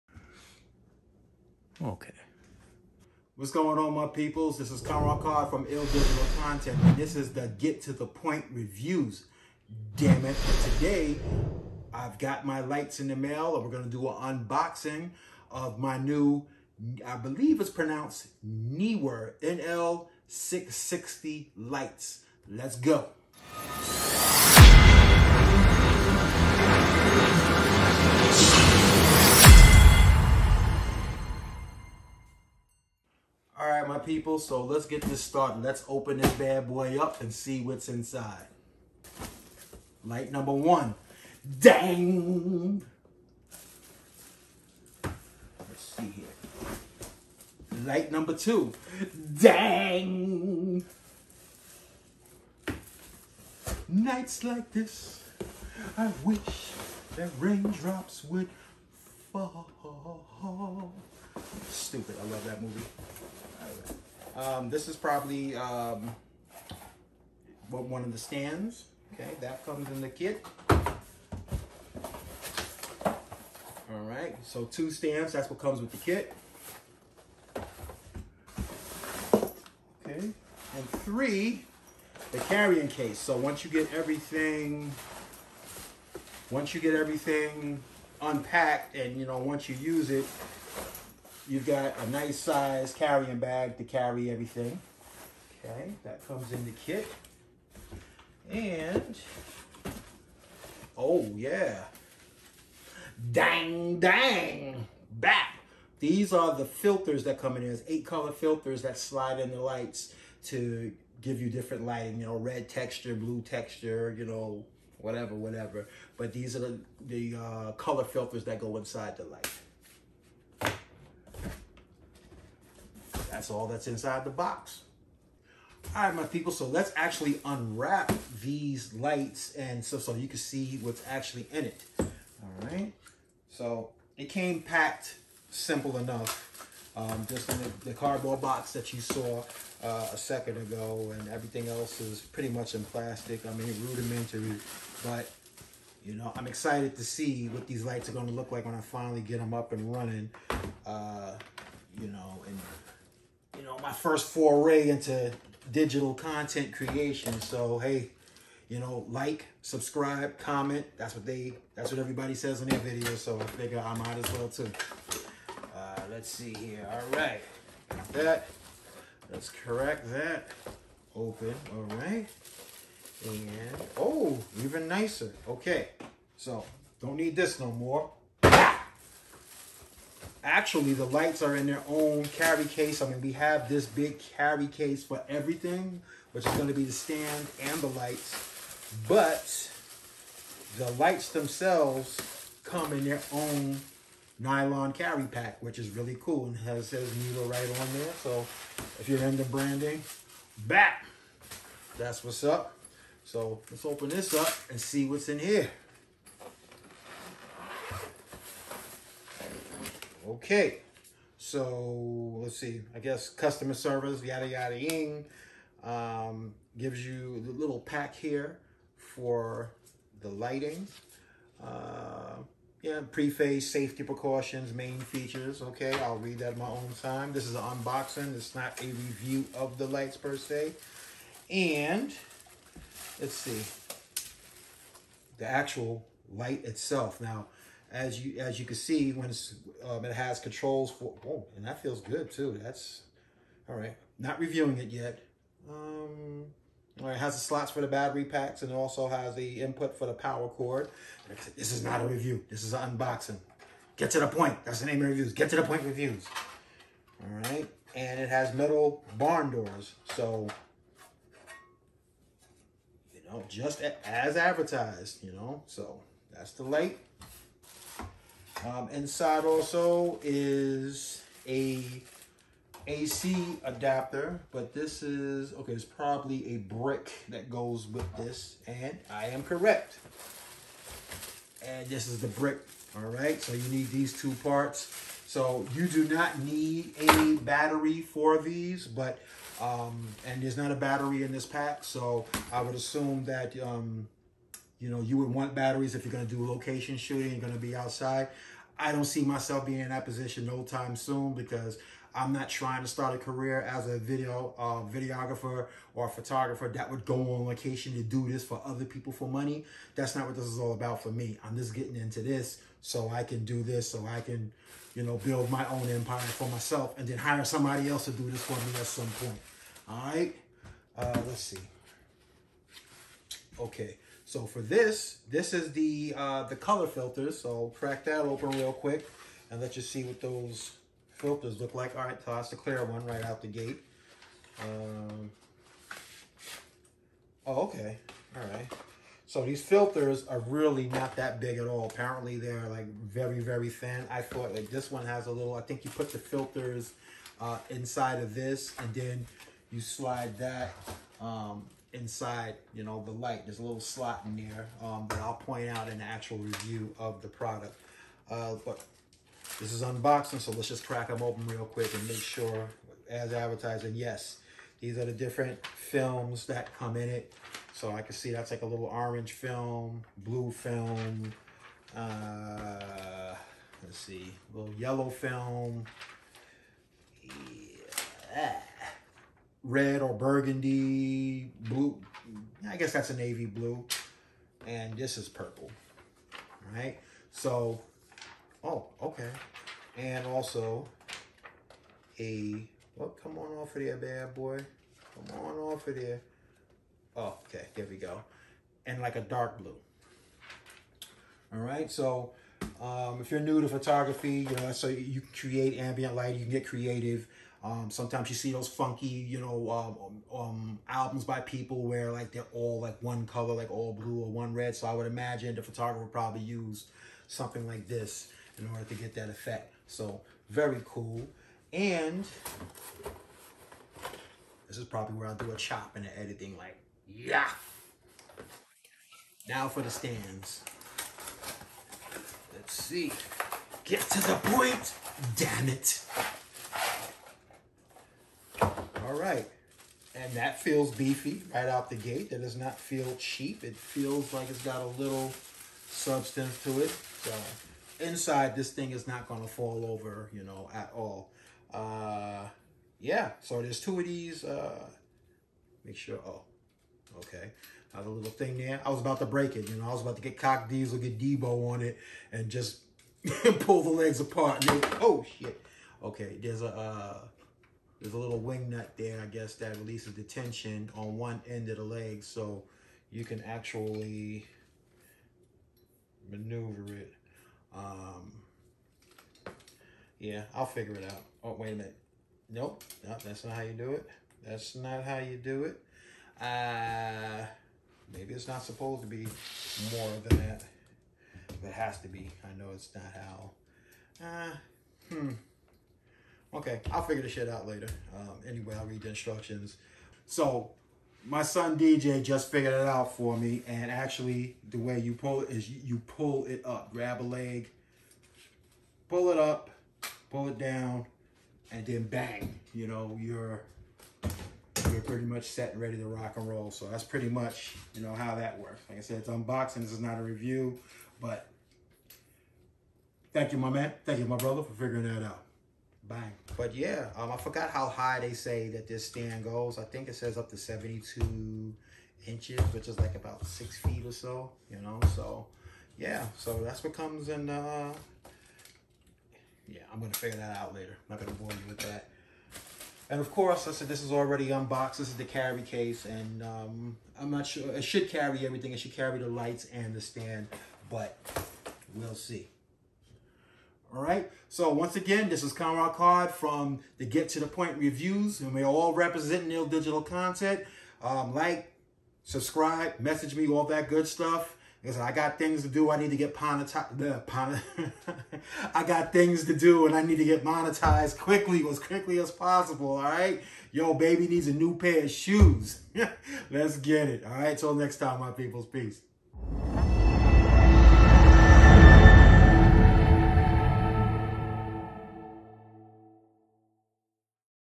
Neewer 660 LED Video and Photography Light - Unboxing
This channel focuses on reviewing FOOD, PRODUCTS and TECHNOLOGY that I use in my personal life everyday.

In this video, I unbox my first set of video and photography lights from NEEWER. These LED Lights were recommended by THINK MEDIA. The review for the NEEWER 660 LED LIGHTS is coming soon.